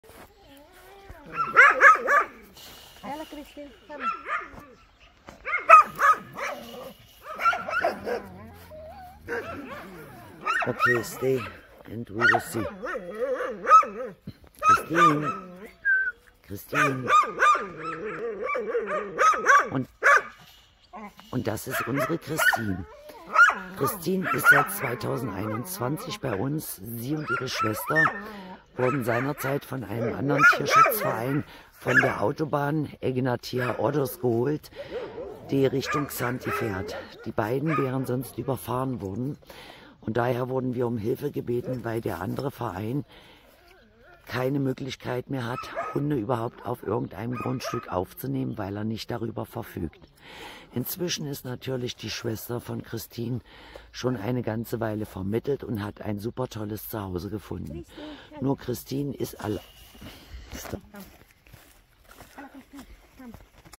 Christine - ein liebenswertes, treues Hundemädchen
Christine und ihre Schwester wurden als Welpen von Tierschützern auf der Egnatia Autobahn Richtung Xanthi gefunden. In größter Not wurden wir gebeten die Welpen aufzunehmen, da es keine Möglichkeit gab, die Kleinen unterzubringen.

Christine und ihre Schwester haben sich trotz der schlimmen Erlebnisse hervorragend entwickelt. Elena, ihre Schwester, hat bereits seit zwei Jahren ihr Glück in Deutschland gefunden.

Unsere Christine wartet leider noch immer, obwohl sie eine sehr freundliche, liebenswerte und verträgliche Hündin ist. Mit unseren Hunden

Christine geht mit ihrer lieben Art, offen auf jeden Menschen und Fremde zu. Sie hat keine Berührungsängste. Besucher werden von ihr schwanzwedelnd empfangen.
Sie wartet brav, bis sie die Aufmerksamkeit erhält und saugt jede Streicheleinheit in sich auf.
Große Kinder kennt und liebt Christine. Sie lässt sich zum spielen animieren und gern von ihnen streicheln.

Sie hat in all der Zeit auch spielerisch gelernt problemlos an der Leine zu laufen.

Katzen oder andere Kleintiere kennt Christine bisher noch nicht.

Für Christine suchen wir eine Familie, welche gern längere Spaziergänge , gern wandert oder sonstige sportliche Aktivitäten unternimmt. Ein Haus mit großem Garten, ländlich oder am Stadtrand wäre ideal.

Christine hat vorher noch nie in einem Haus mit Menschen gelebt, daher sollte die Familie entsprechend Geduld und Verständnis haben.

Geschlecht: Hündin
Rasse:        Mischling
Farbe:         weiß braun
Alter:         ca. 02/2021
Größe:        55 cm
Status: geimpft, gechipt, kastriert, MMK-Test, EU-Pass
Aufenthalt:  Griechenland

Wenn Sie mehr über Christine erfahren, oder ihr ein liebevolles Zuhause für immer schenken möchten, kontaktieren Sie uns gern.